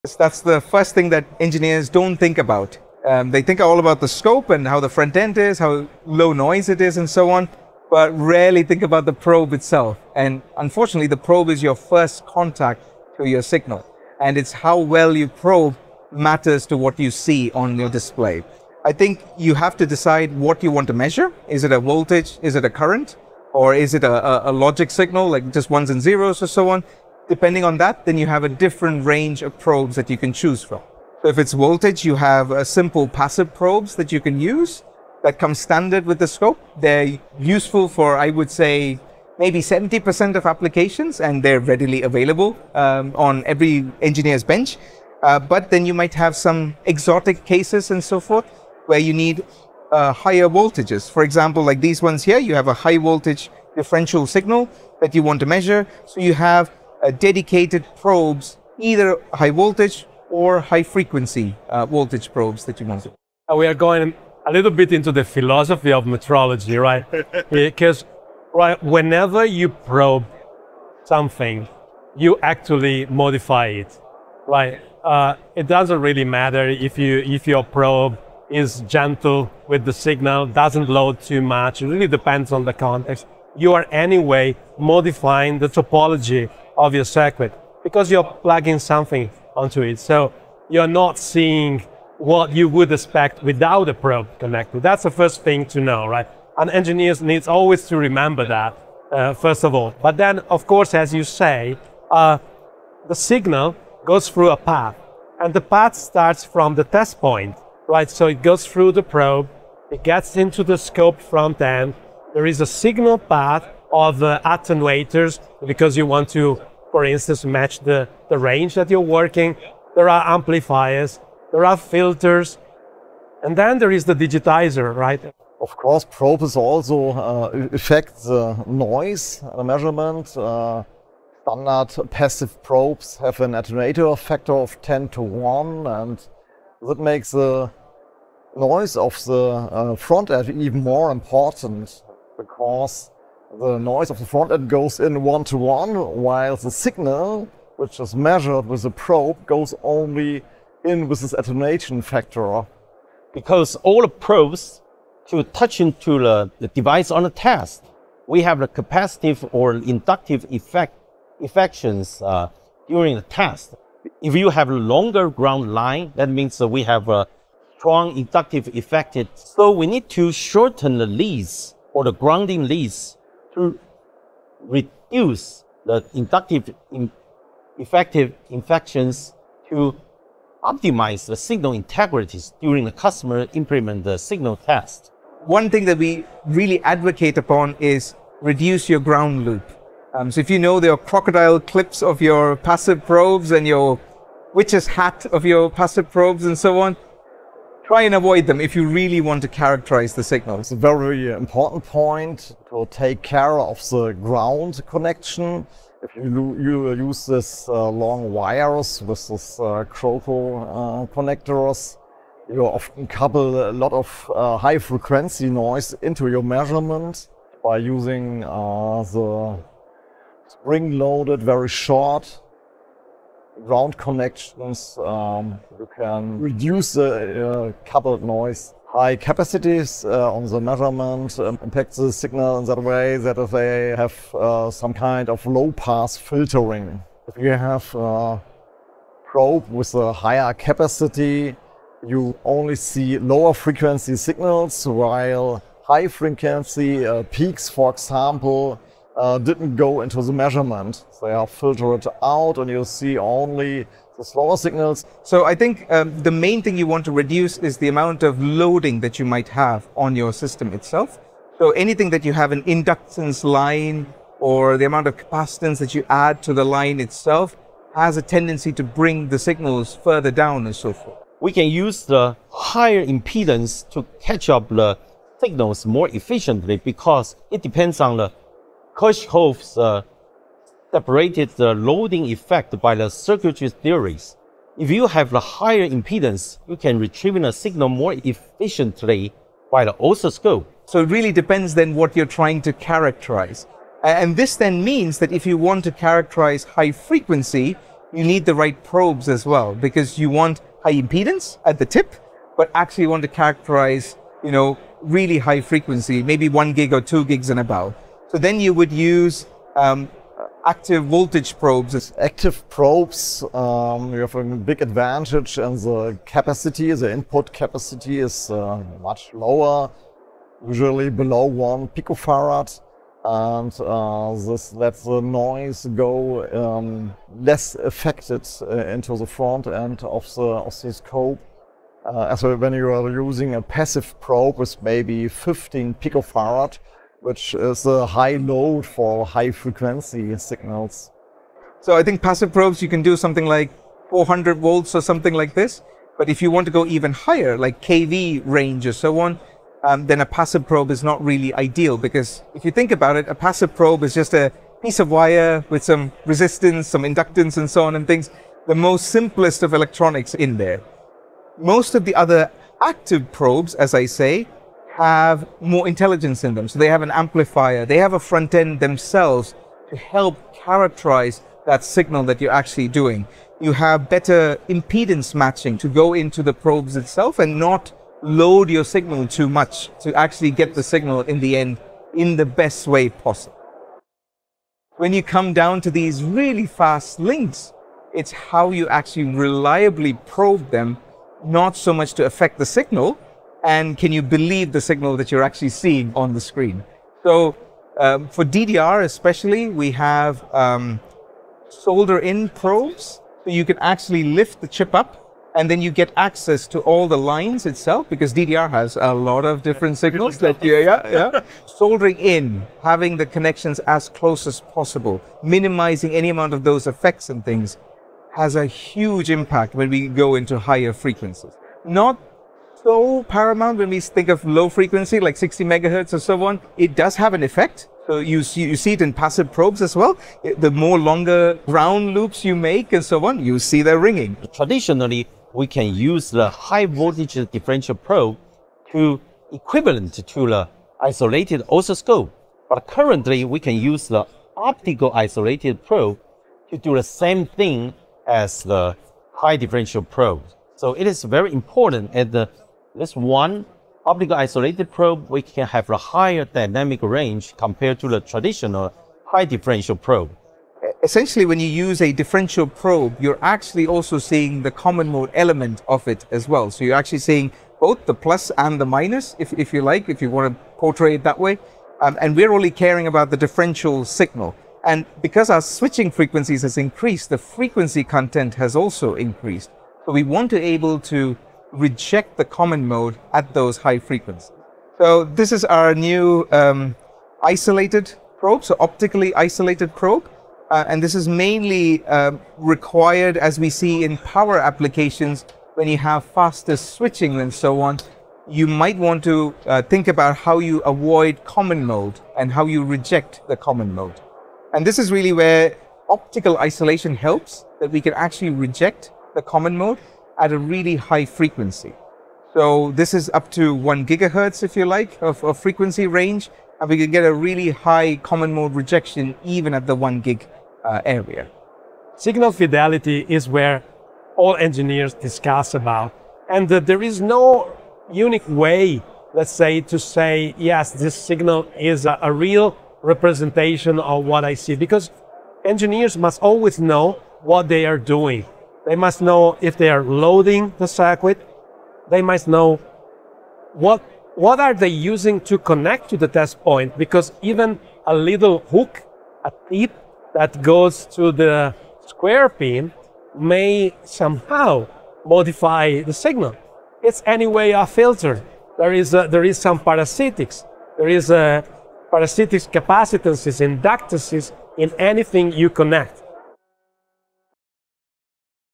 Get the Most From Your Oscilloscope Probes: Expert Advice
In this comprehensive guide to oscilloscope probing, industry experts from Rohde & Schwarz, Tektronix, Rigol, and Batronix share their invaluable insights on selecting and using the right probes for your measurements.
Learn why the humble probe is perhaps the most critical yet overlooked component in your measurement chain. Our experts discuss:

How to choose the right probe for your specific measurement needs
The crucial differences between passive and active probes
Why ground connections dramatically impact your signal integrity
Advanced techniques for high-frequency measurements
The importance of impedance matching and signal loading
Specialized probing solutions for DDR and power applications
How optical isolation solves common mode rejection challenges

Whether you're debugging a simple circuit or characterizing high-speed digital signals, this video will help you avoid common probing mistakes and achieve more accurate measurements. Remember: your measurement is only as good as your probe!